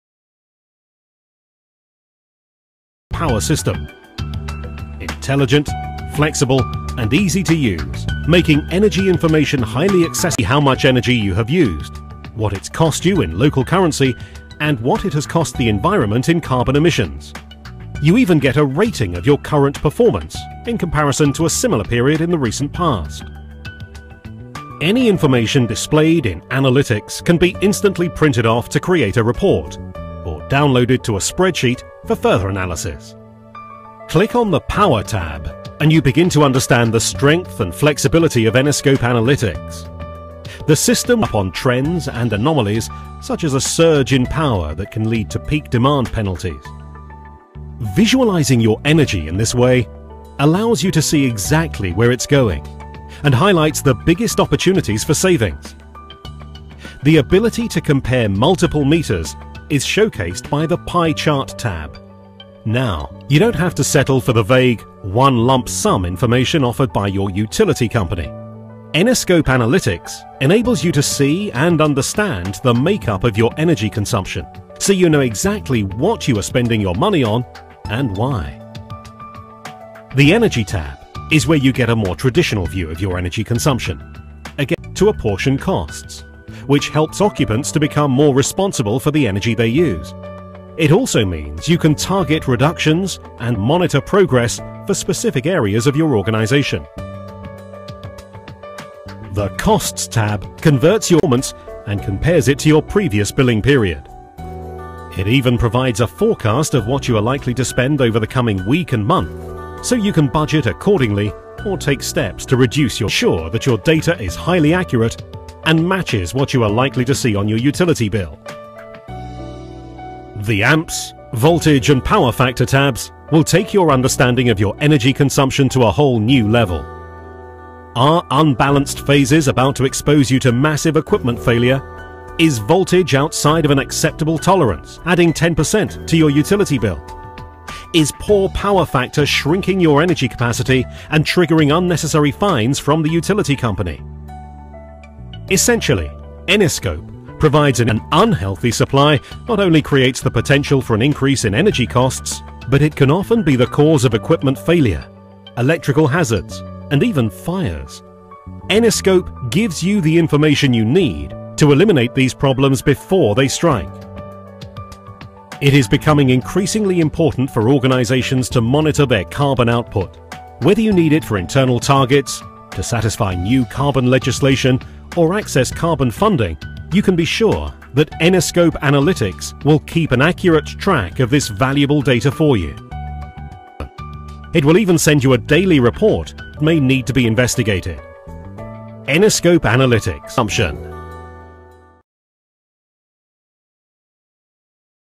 Eniscope Analytics SMARTech Energy
Did you know that up to 43% of global energy consumption is going to waste as a result of inefficient equipment and business practices? How much energy is going to waste in your business? Do you know where, when and how your energy is consumed? Find out how SMARTech Energy can help you